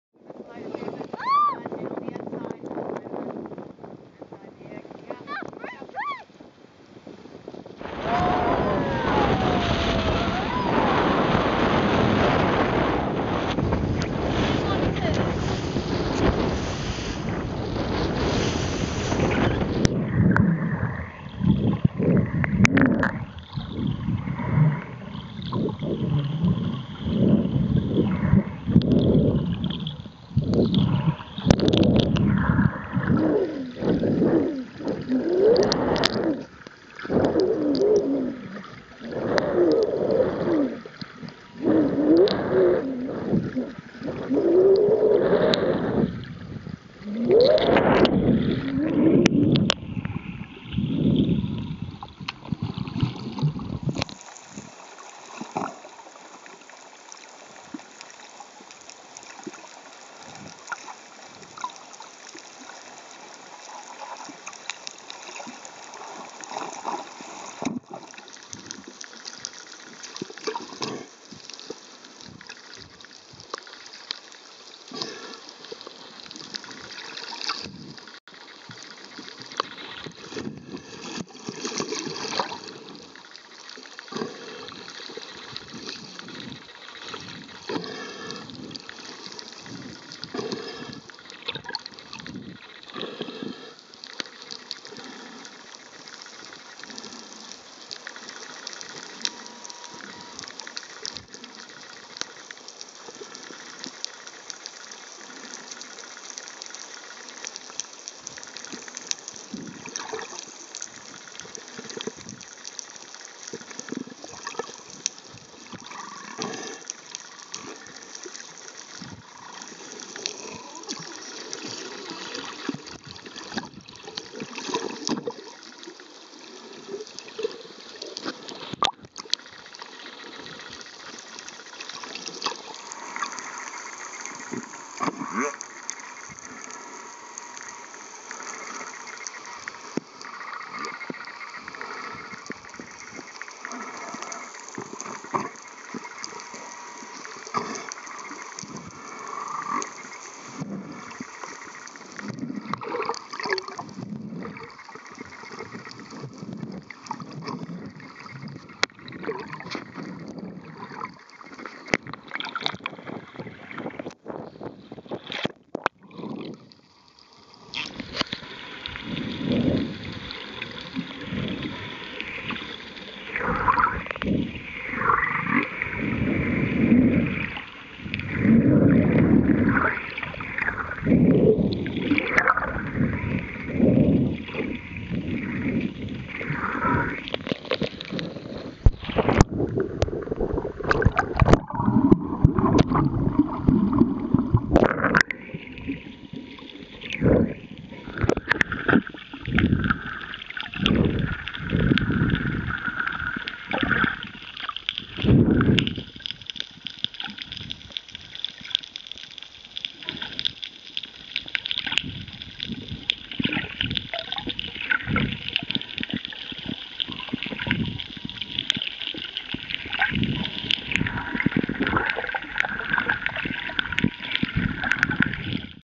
Tonga - Vava'u Whales - Barbara Kendall
Swimming with the Whales in Vava'u with the great people with Mounu Island. Barbara Kendall